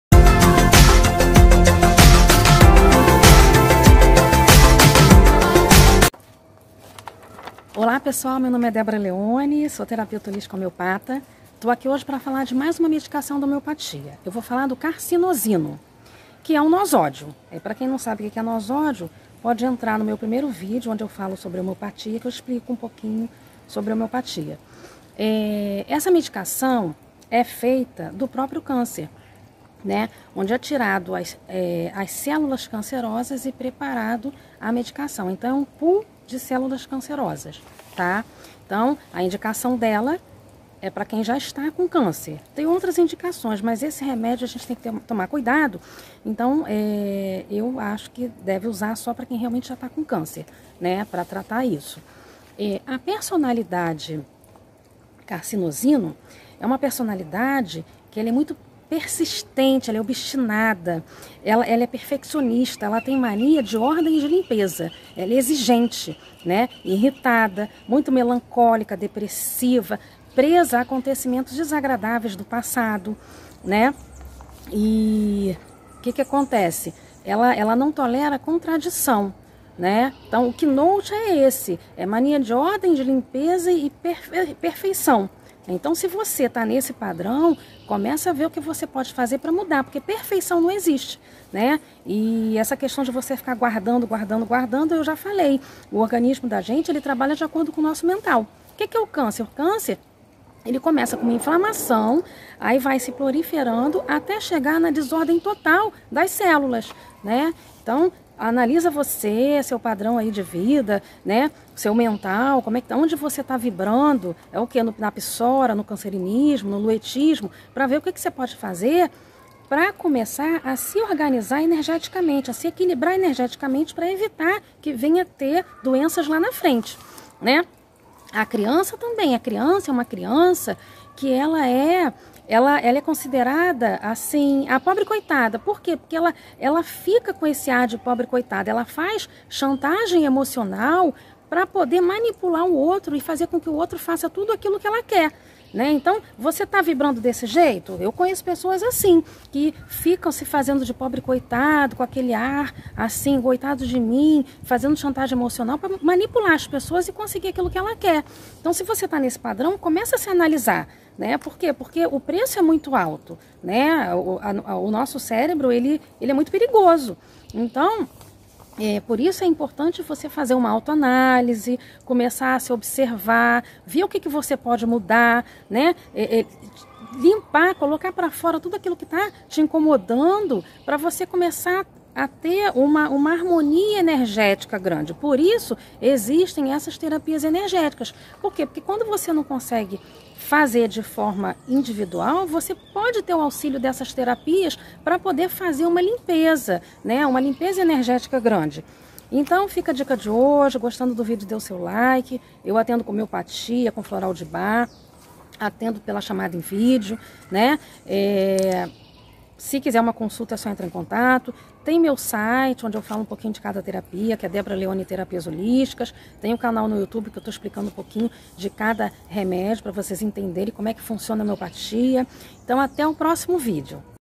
Carcinosinum nosódio feito de células cancerosas
Carcinocinum nosódio feito de células cancerosas
Neste video eu explico sobre a homeopatia da carcinosinum e seus beneficios

Atendimentos on line: Homeopatia, Baralho Cigano, Cura Multidimensional Arcturiana, Floral de Bach, Cura Planetaria Cósmica, Reiki, Cristalterapia, Acupuntura Etérica Quantica.